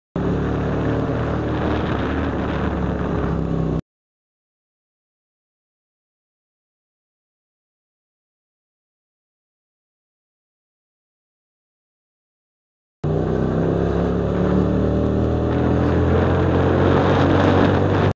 Old couple in middle of road
Driving around the Maltese roads on a 2013 Hyosung GT250R with a 808#16 V2 camera mounted inside my helmet.